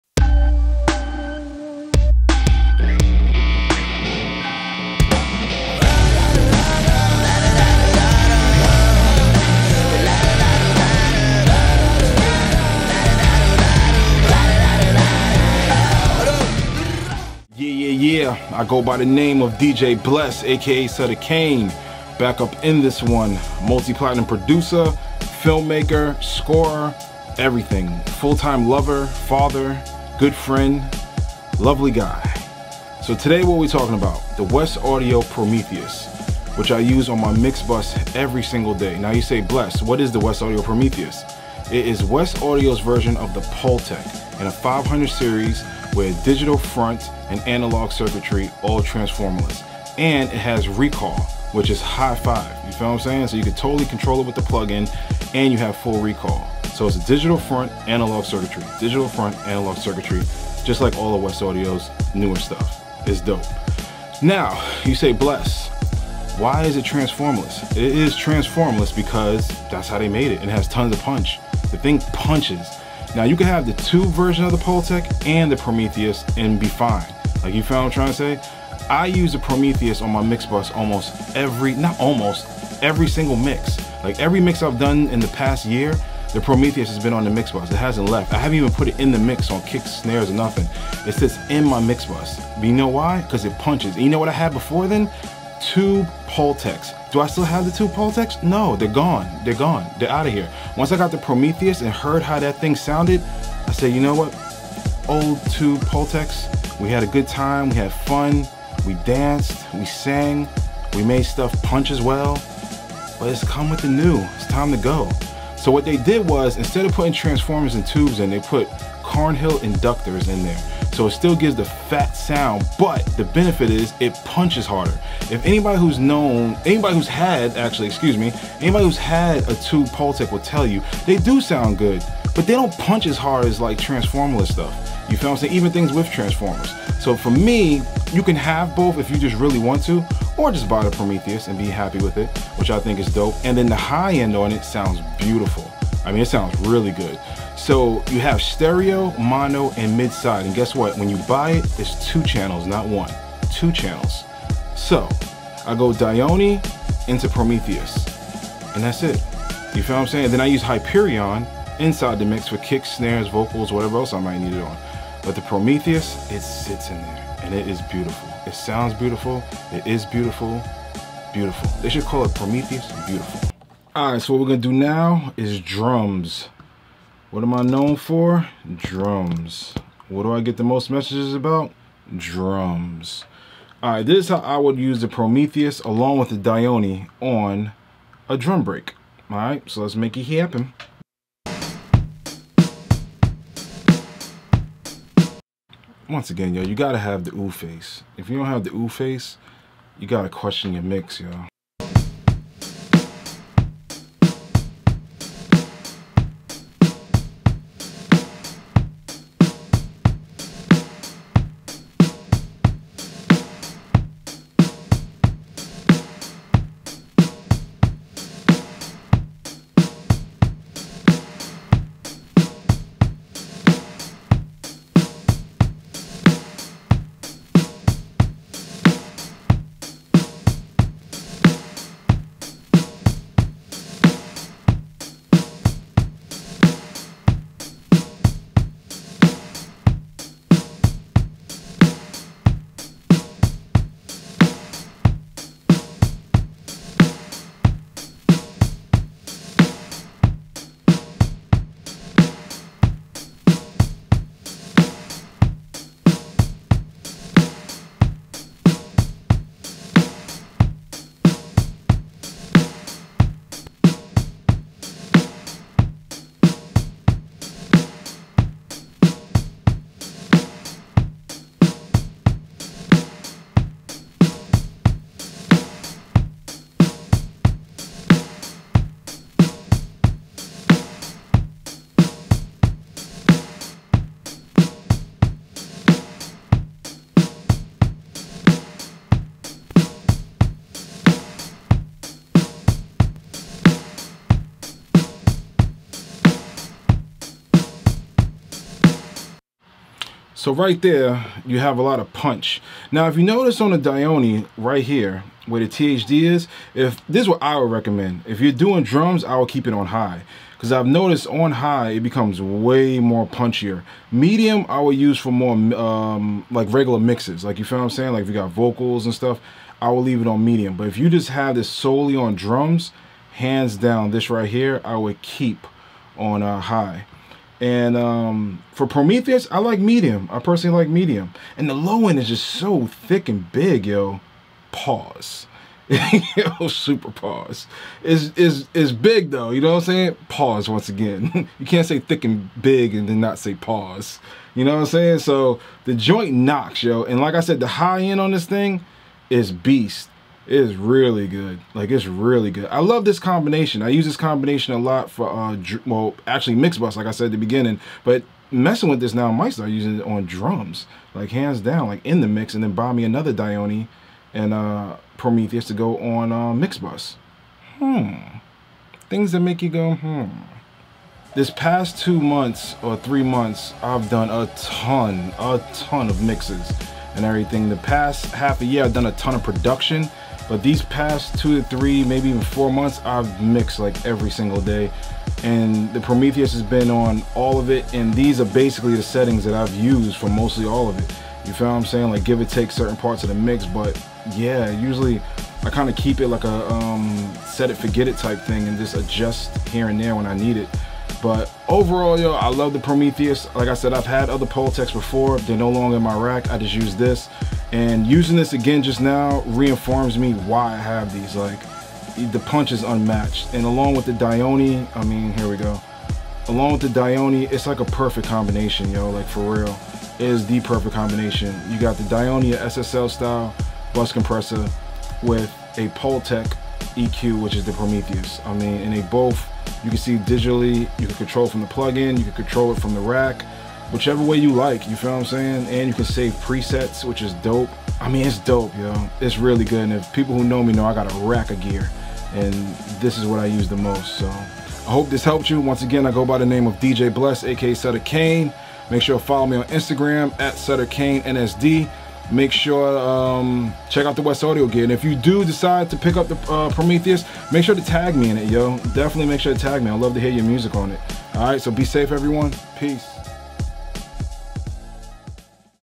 DJ Bless aka "Sutter Kain" about WesAudio _PROMETHEUS
DJ Bless talks about _PROMETHEUS - stereo/dual mono/mid side passive analog EQ with digital control.